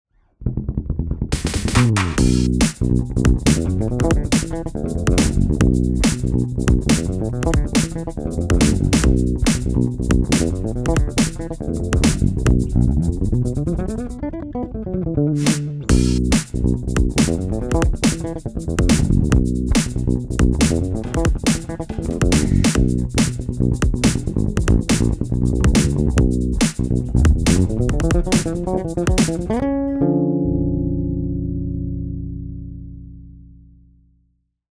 fast funky BASS groove!
Here is another jam in Bm. What else do you need?
Keep an eye on the ghost-notes, they should be as funky as possible 🤤.

//FEEL FREE TO VISIT!

Thanx a lot for your support folks.